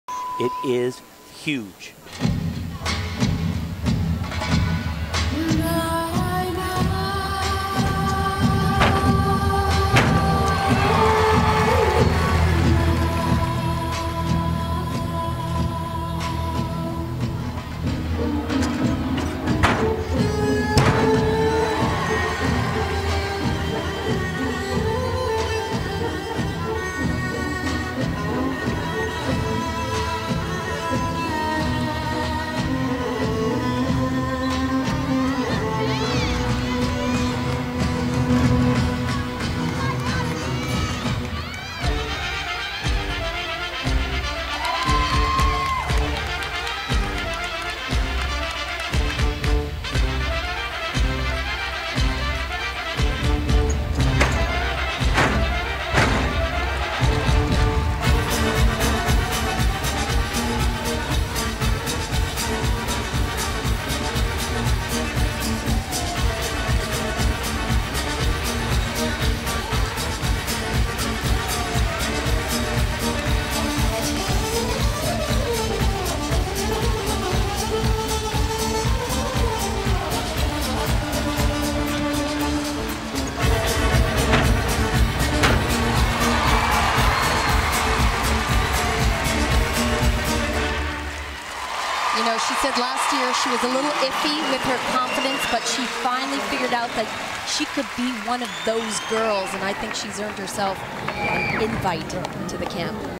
Kytra Hunter - Floor Exercise - 2010 Visa Championships - Women - Day 2
Score - 14.800 - Aug. 14, 2010 - XL Center, Hartford, Conn.